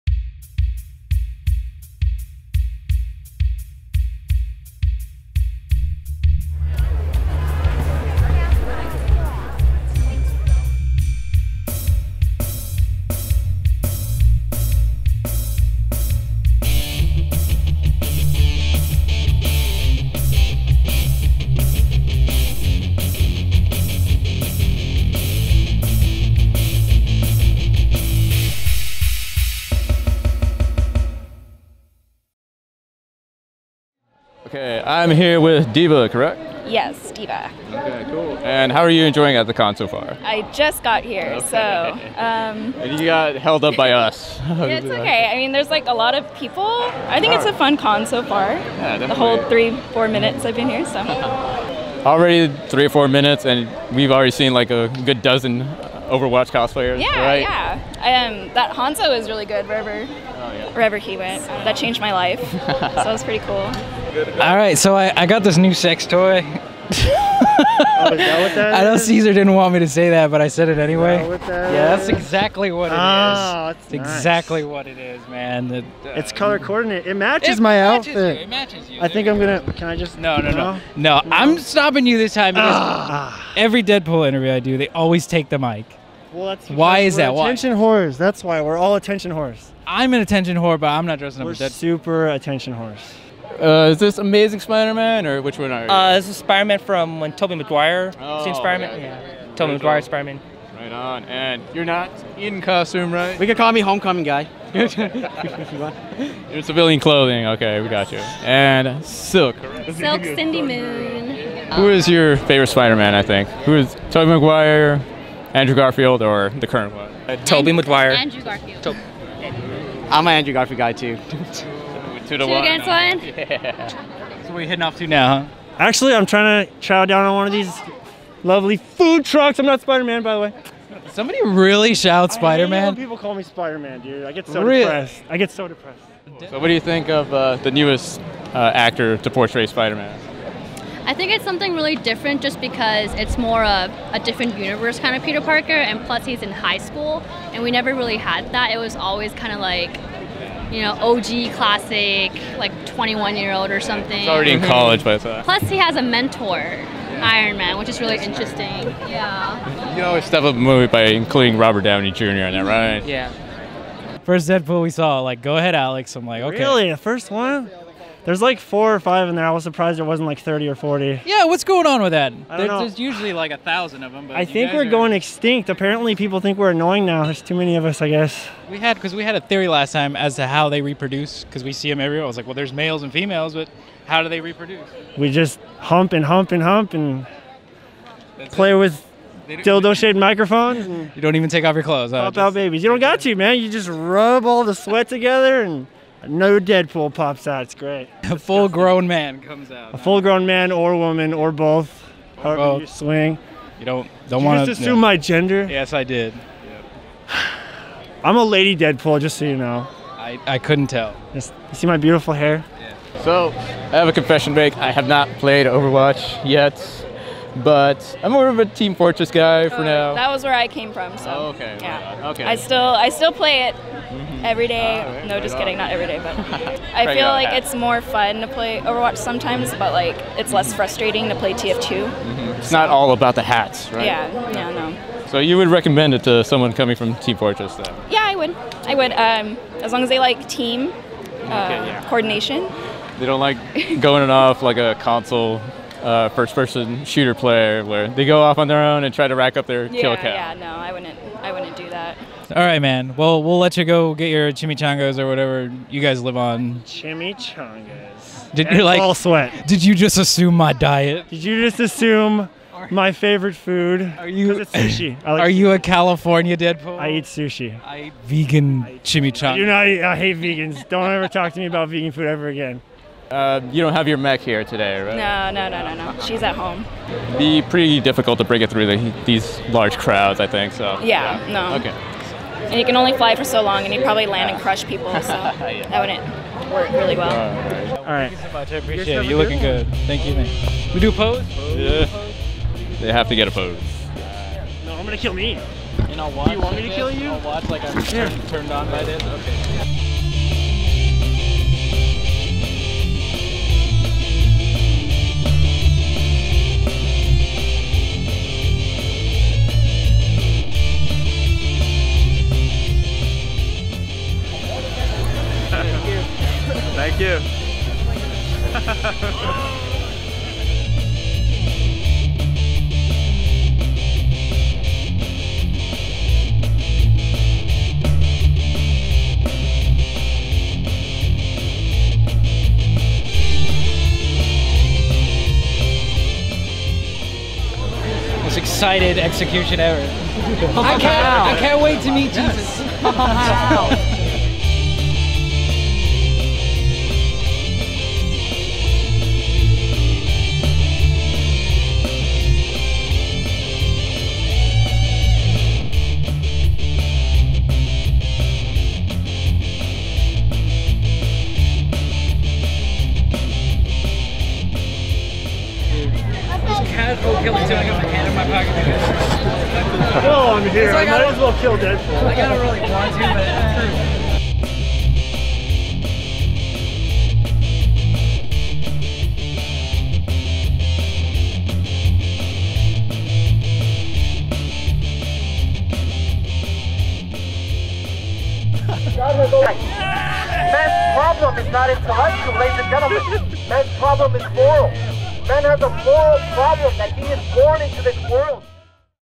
Wondercon 2017 4k Interviews Part One - Conman Cosplay
Three weeks later...

4K cosplay interviews are here! The second video shouldn't take so long now since I can recycle settings from my first video. The third and fourth cosplay videos however...

Be sure to show all of the cosplayers some love. Thank you guys for participating!

Black Cat -